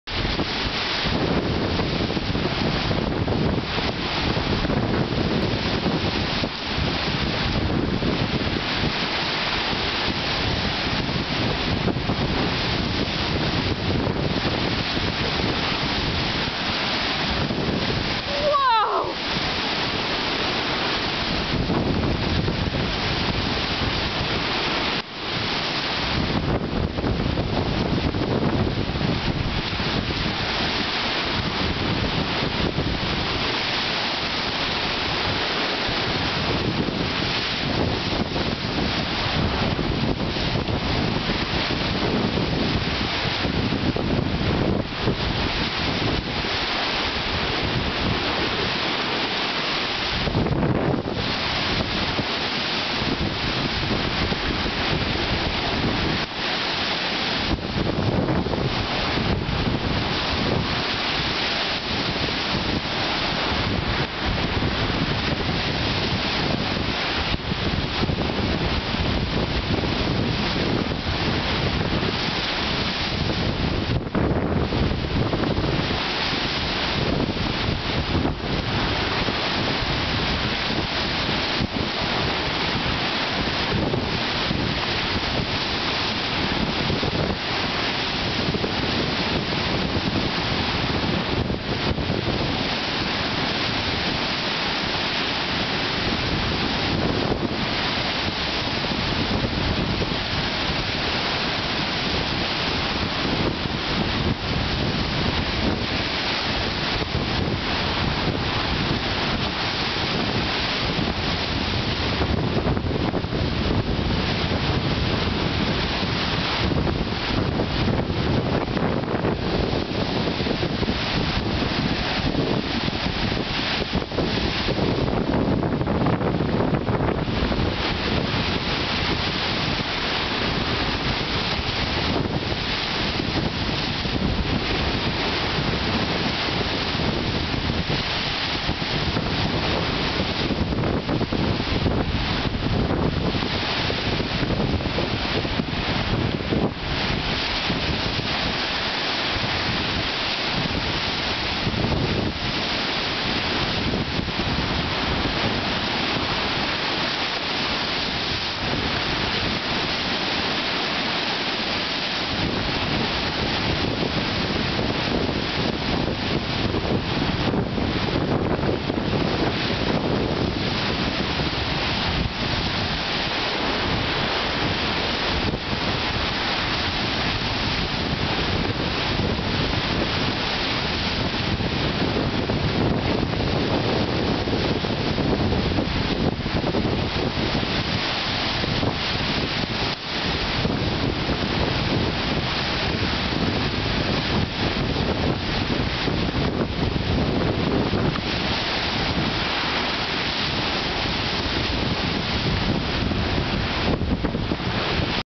Flathead Lake Wind and Waves
We had wild wind today - Mike went down to secure the boat lift motor - since we lost the last one in similar weather in 07. He got NAILED with waves.